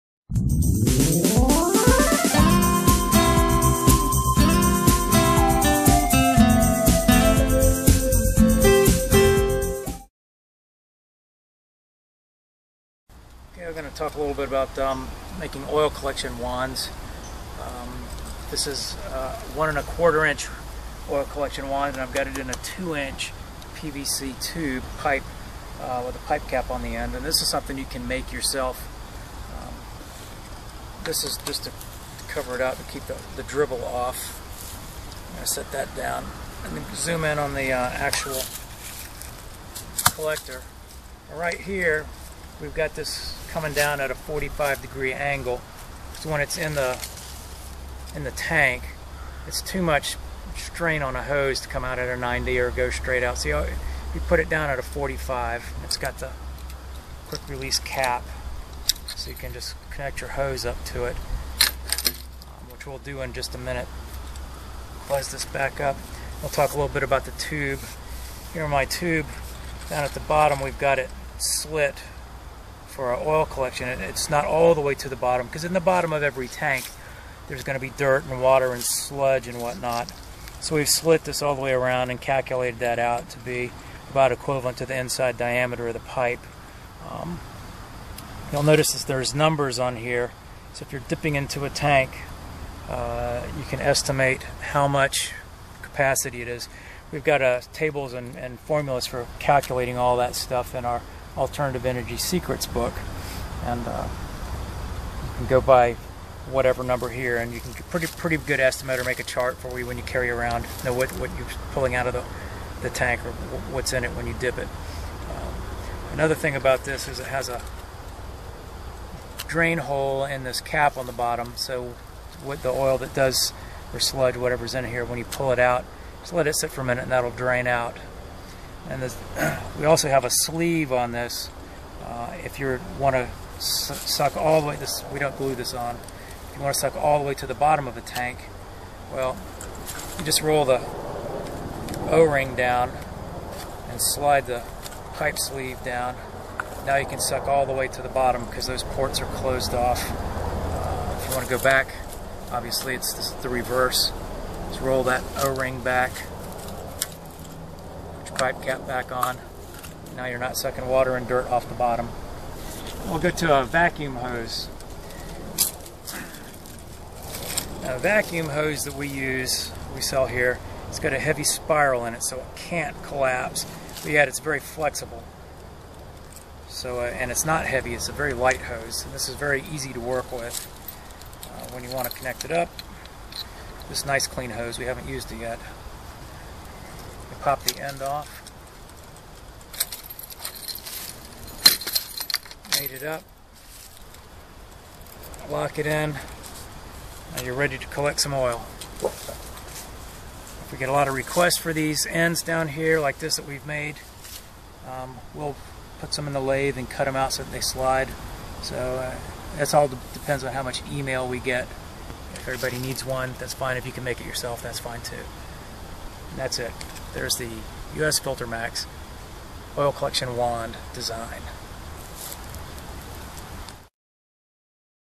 Make an Oil Collection Wand For WVO, Used Oil and Motor Oil
Short Description of an oil collection wand for use with the vacuum trailer or with a gear pump.